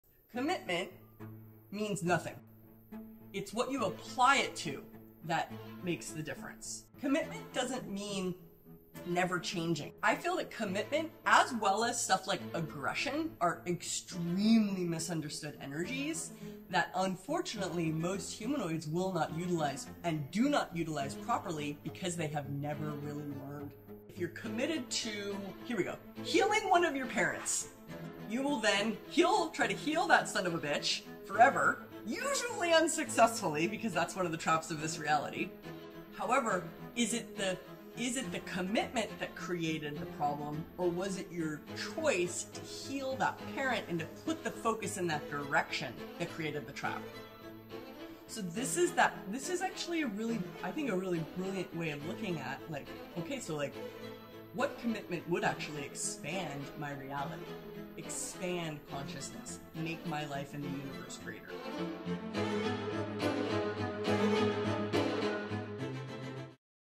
Commitment Club Consciousness with Shannon O'Hara
What can you be committed to now ?

What commitment would actually expand your reality , would expand Consiousness ?

What commitment would actually make your life greater ?

Join Club Consciousness and deeply expand your life .

Next Call on March 24th